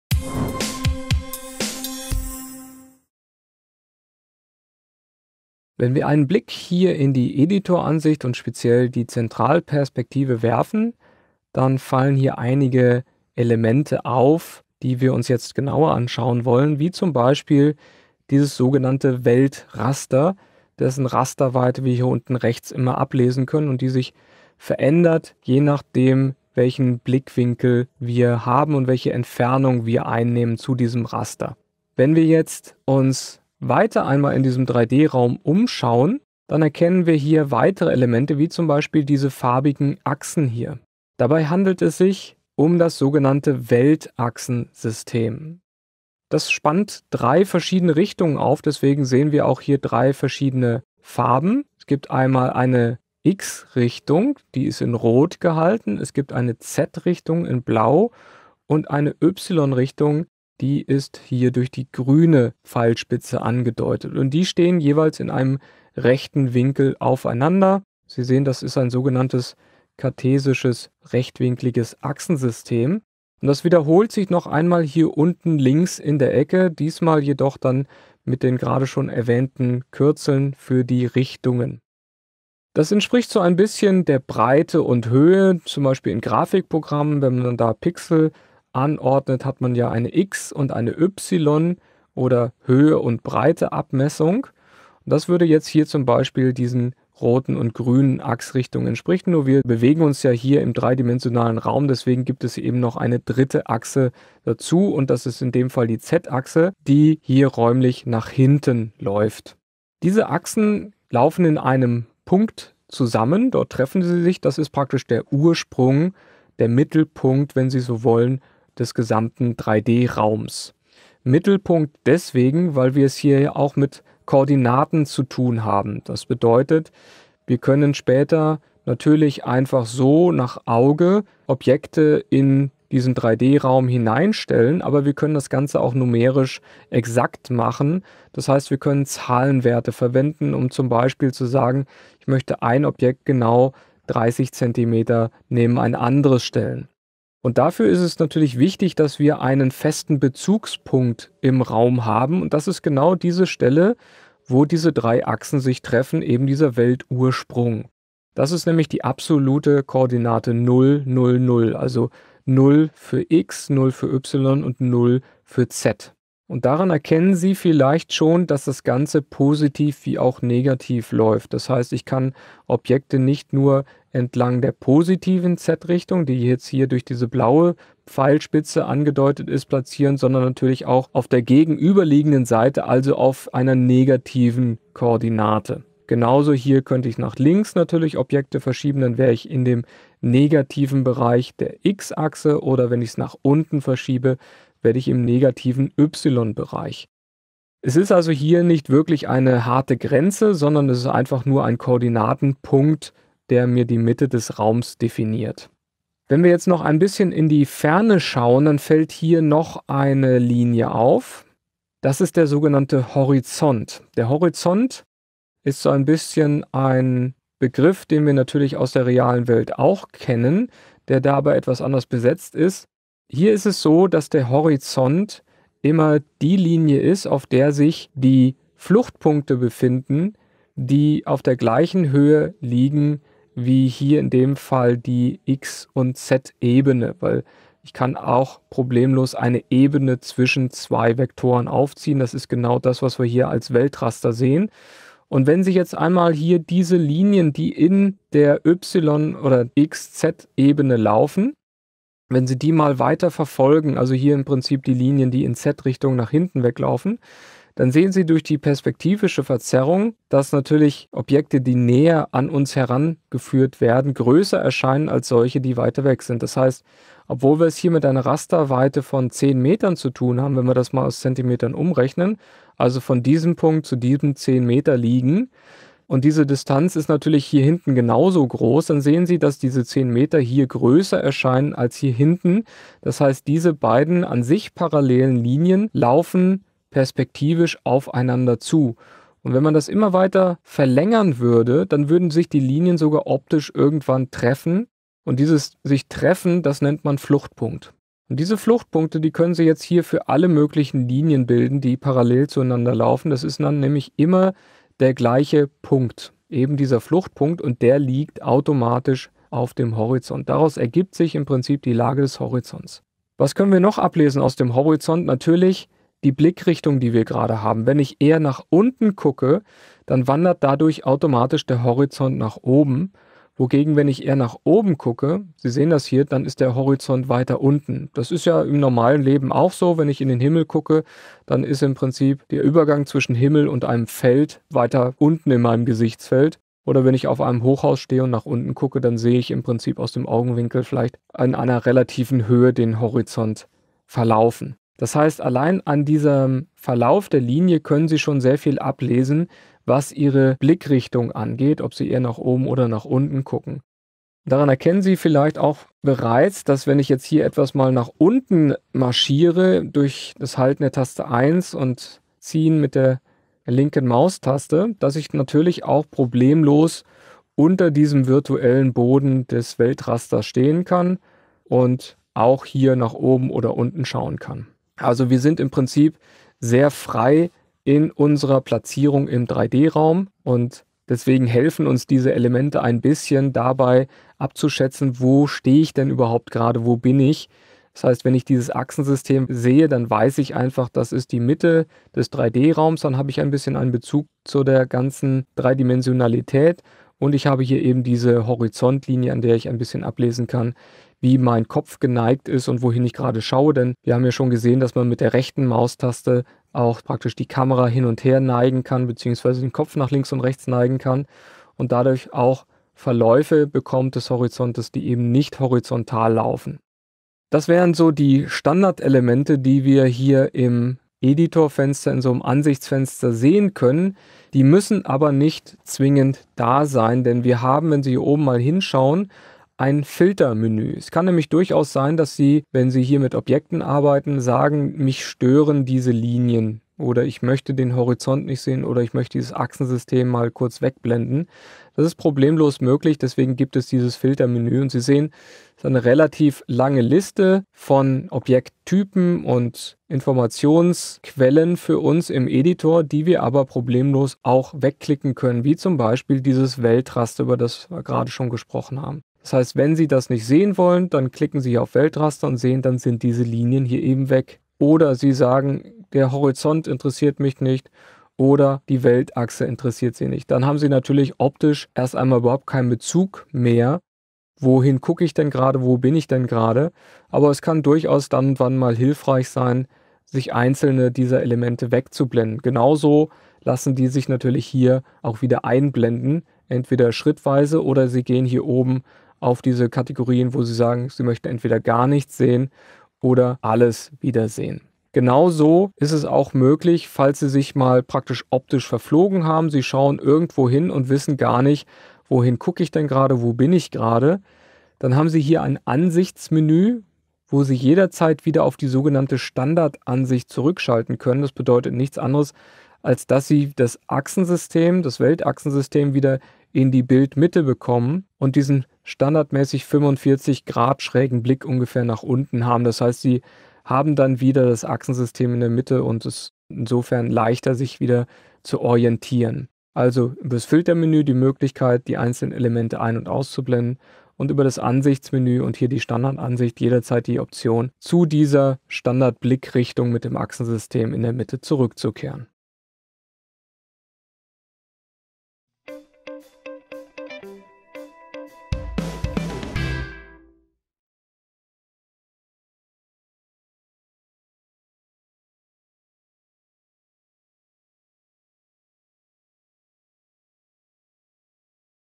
Cinema 4D Tutorial: Symbole und Elemente einer Editoransicht |video2brain.com
Komplettes Video-Training Das Weltraster, das Weltachsensystem und der Horizont erleichtern die Navigation im 3D-Raum. Dennoch lassen sich diese Elemente auch ausblenden, sollten sie optisch stören.
Dieses Tutorial ist ein Auszug aus Arndt von Koenigsmarcks neuem Video-Training „Cinema 4D – Grundlagen. Ihr umfassender Einstieg - geeignet ab Version 16.“